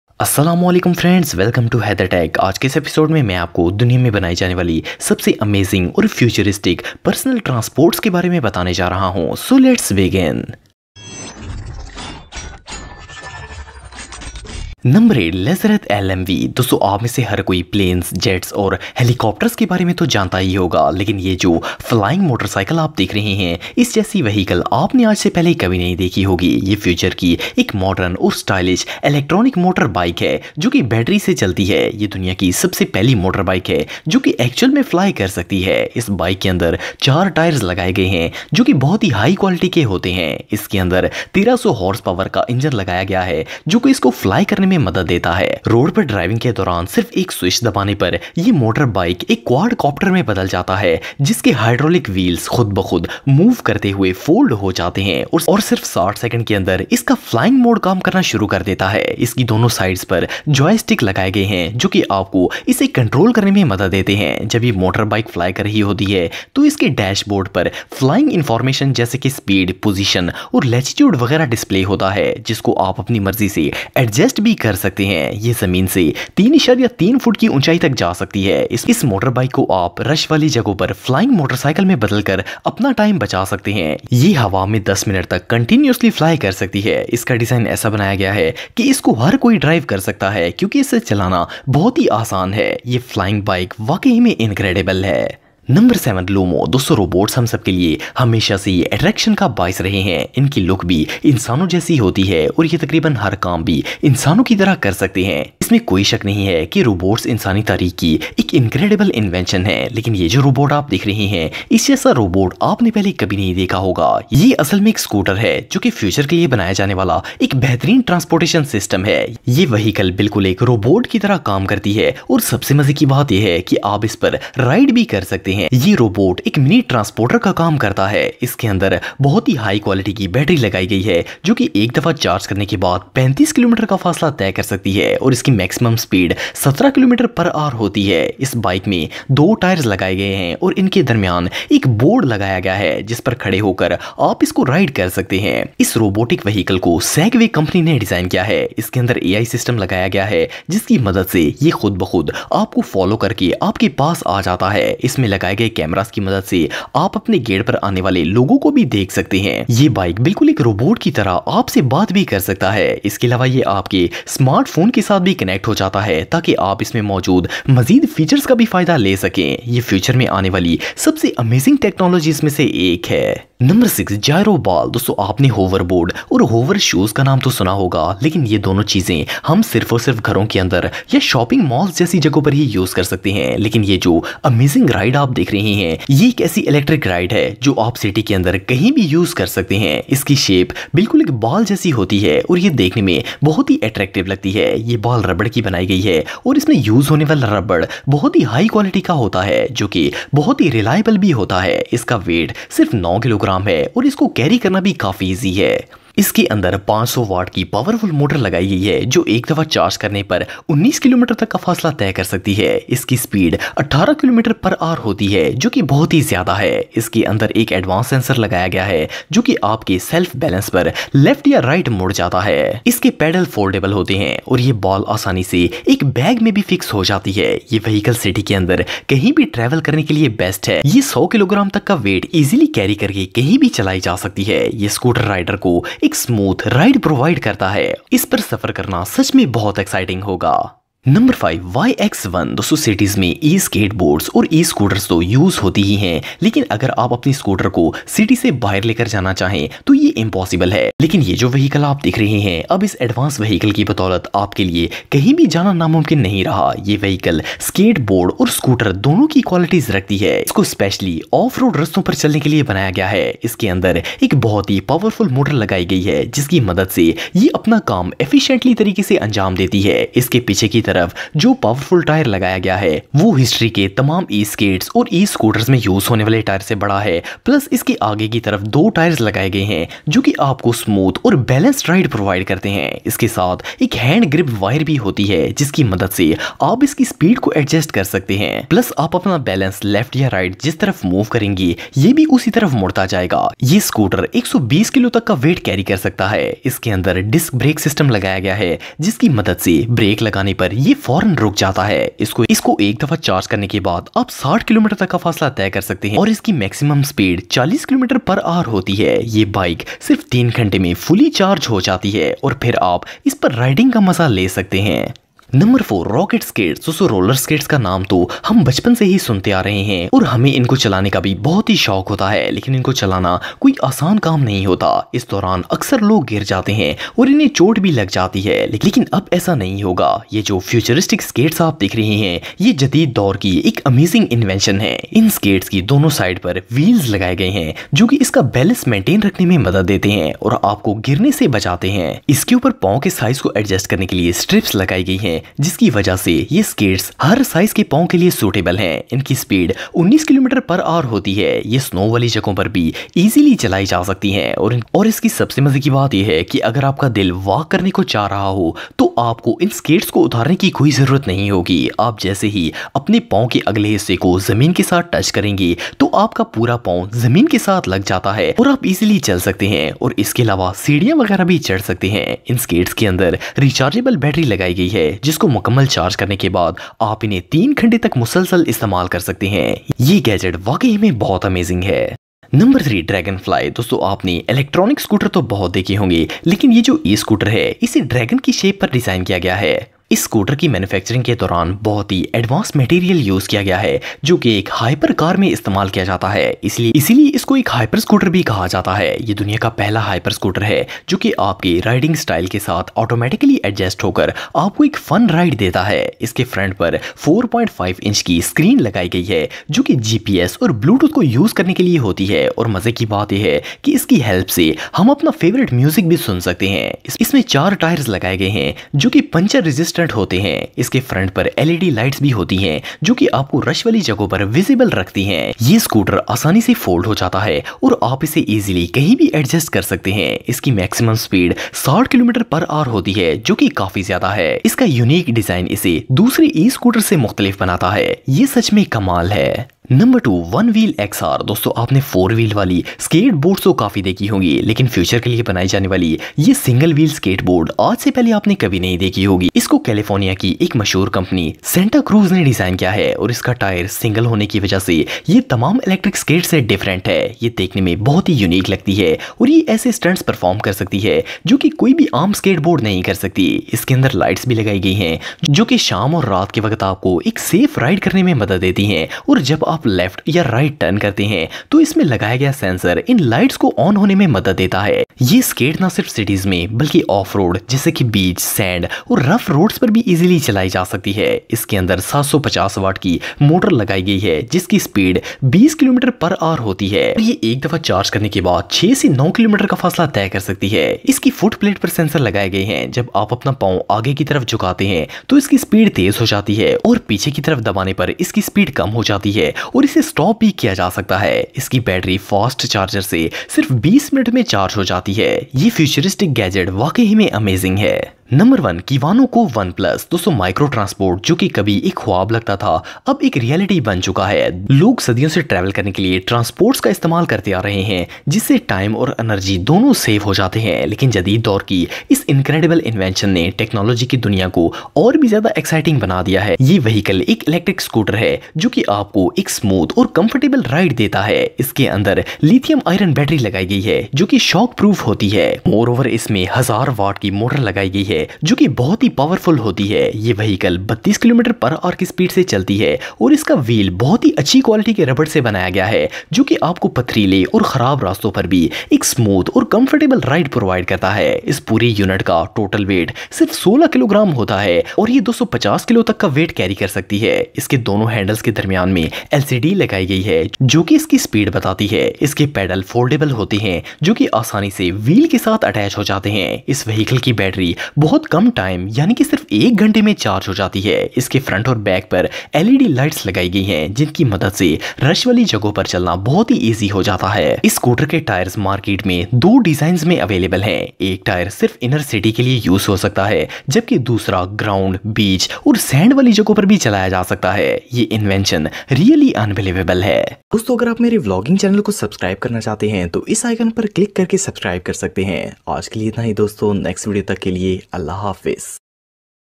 Dunya Me Mojood 8 Sabse Advance Personal Vehicles | Amazing vehicles | Haider Tech
Asalm O Alikum haidertech Family. In This Video I Will Tell You About Most Advance Personal Vehicles Hope You Guys Enjoy The Video Give A Like If You Enjoy The Video And Also Subscribe To My Channel To Get More Inormative Videos Like This. Take Care Allah Hafiz.